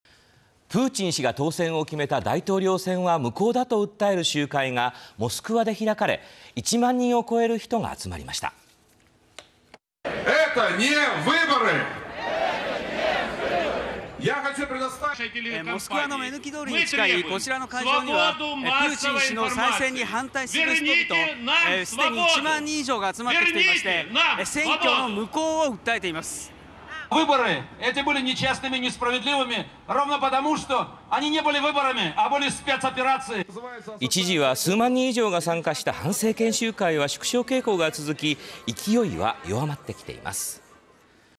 反プーチン派集会に1万人 大統領選の無効訴え（12/03/11）
プーチン氏が当選を決めた大統領選は無効だと訴える集会がモスクワで開かれ、1万人を超える人が集まりました。